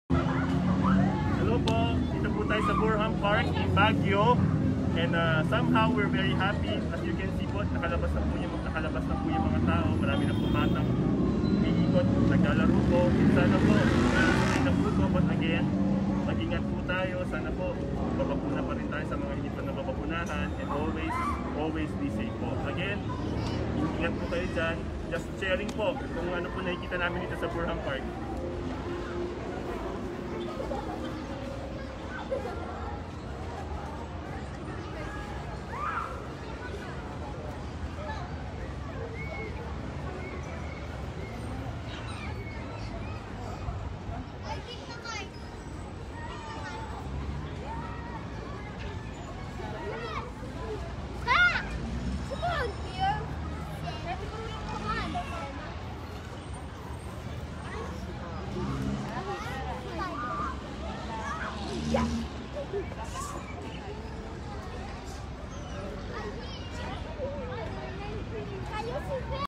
Dito po tayo sa 📍Burnham Park! (November 20, 2021)
Dito po tayo sa 📍Burnham Park! Bukas na bukas na po talaga ang Baguio. Marami na po turistang sabik na mamasyal. Palagi po tayong mag-iingat 🙏🏼